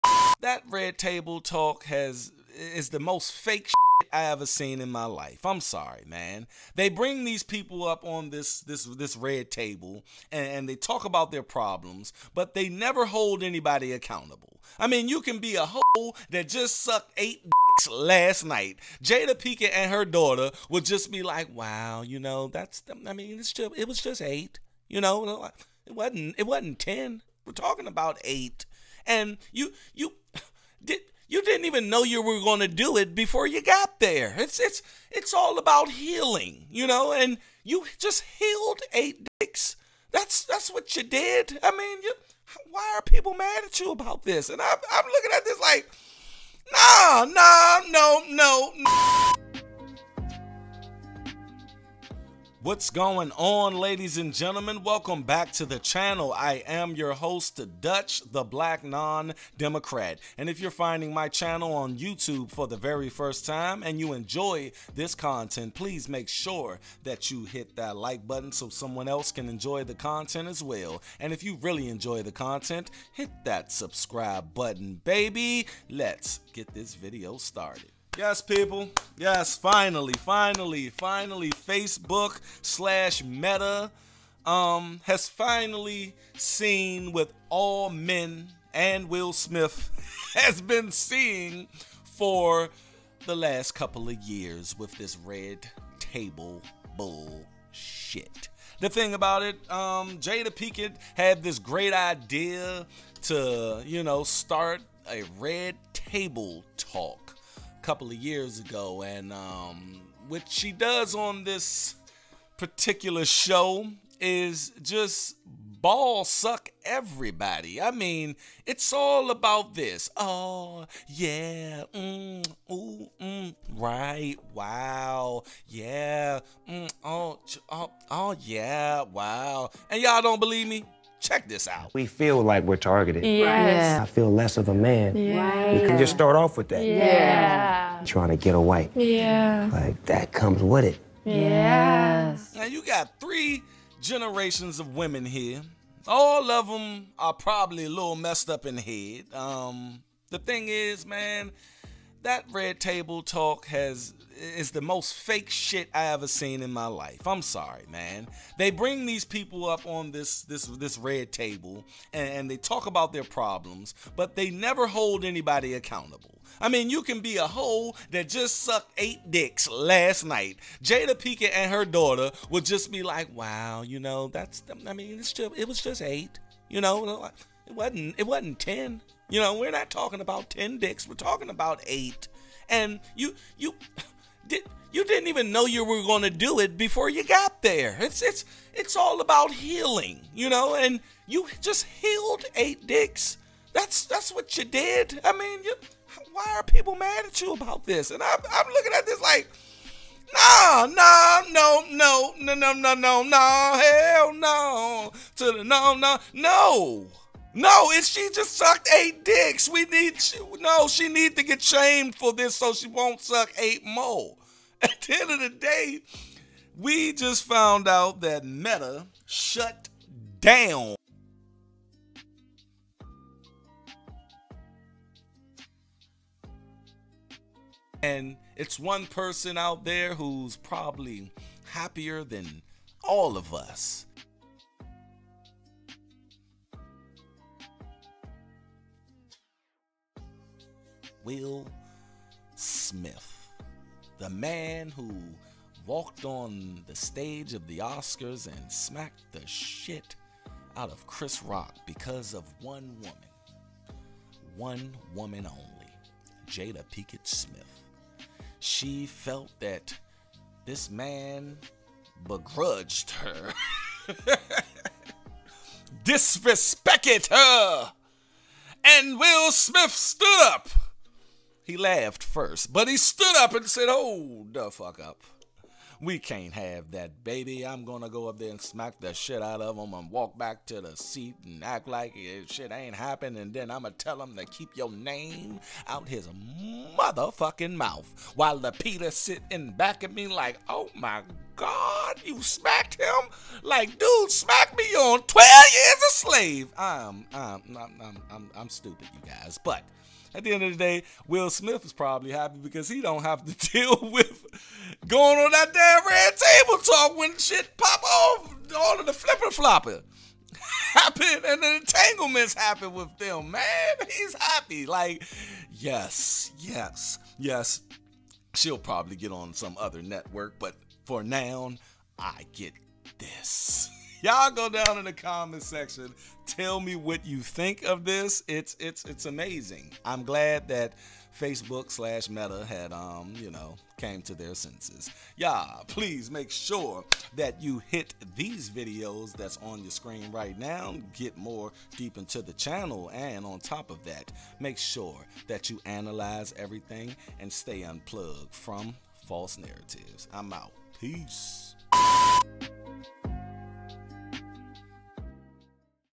Jada Pinkett's "RED TABLE TALK" Finally Gets Cancelled By Facebook & Will Smith Loves It!
THE RED TABLE TALK HAD BEEN CANCELLED BY FACEBOOK | On today's episode Dutch reacts to the news that Jada Pinkett's RED TABLE TALK had been cancelled by Facebook.